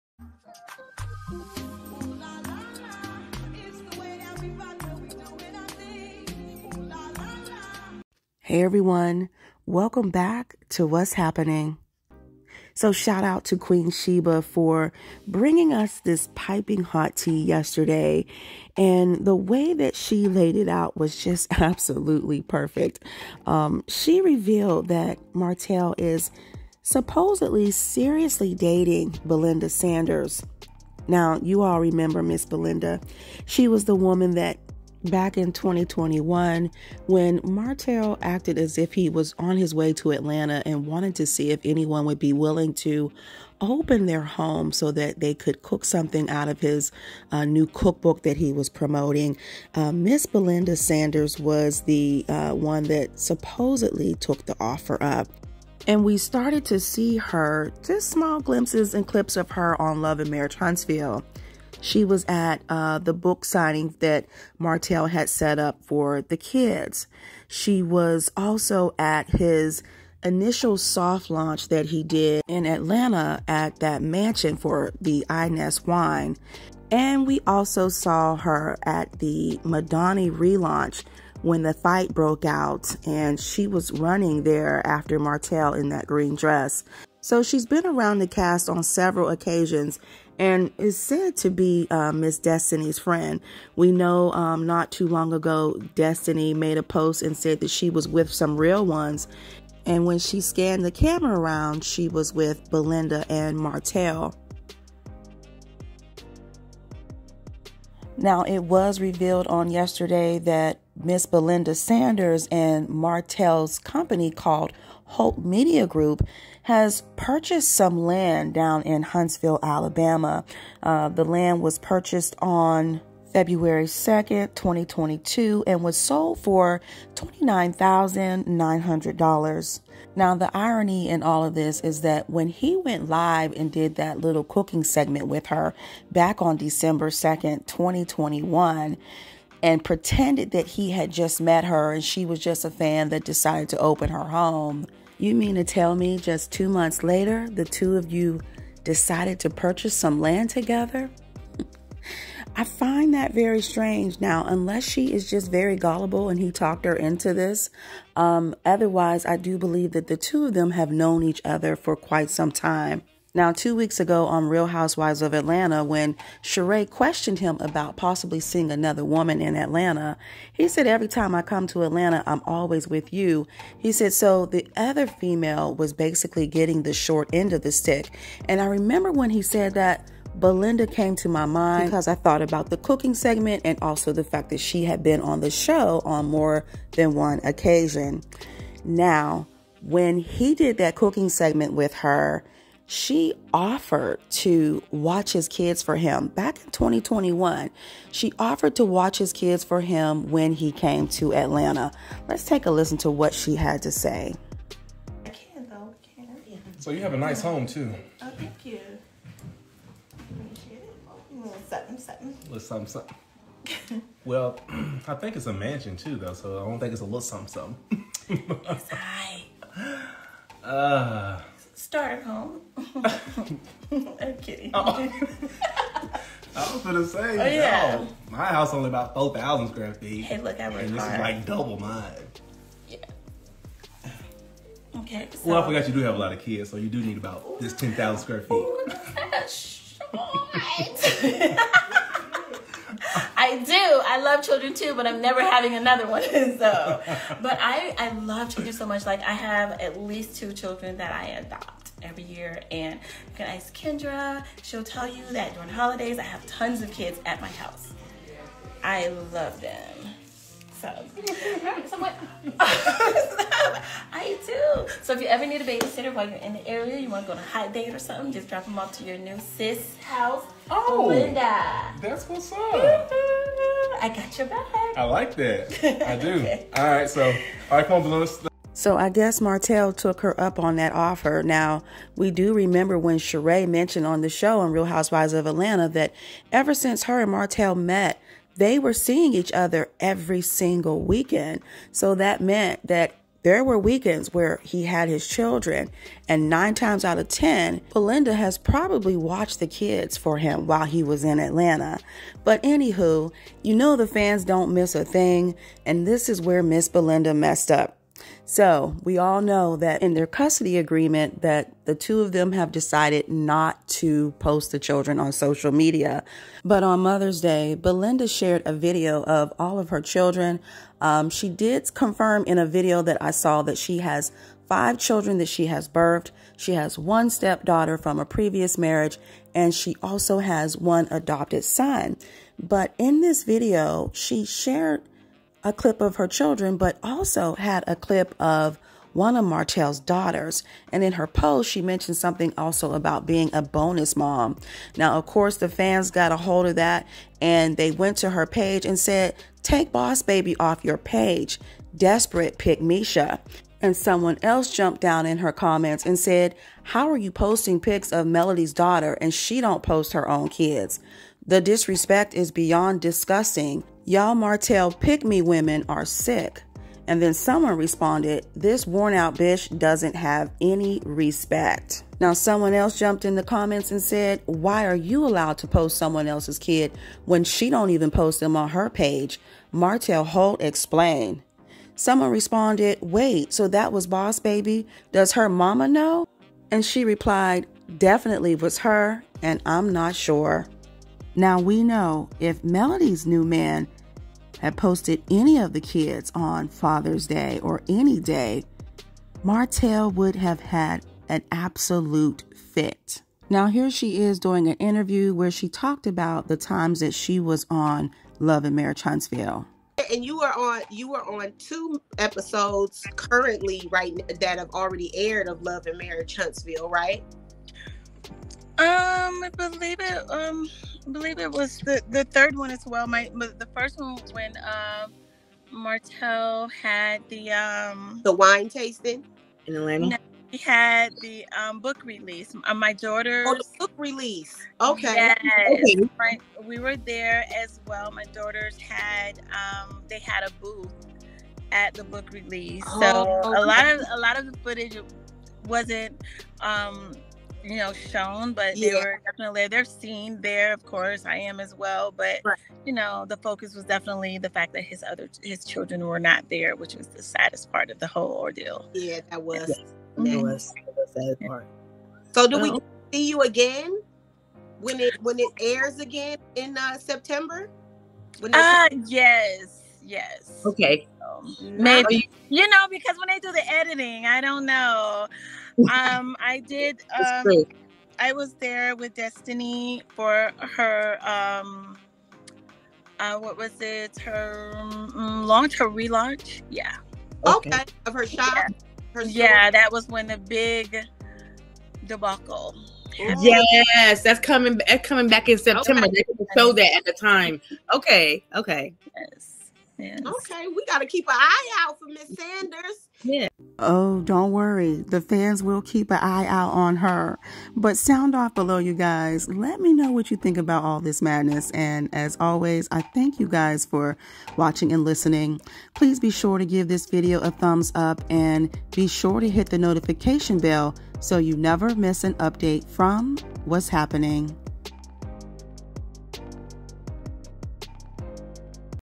MARTELL HOLT'S OTHER WOMAN (BELINDA) POSTED A PICTURE OF HIS DAUGHTER!! WHAT'S HAPPENING
Martell Holt's alleged other woman, Belinda posted a picture of his daughter with Melody Shari all while Melody is not allowed to post her own children.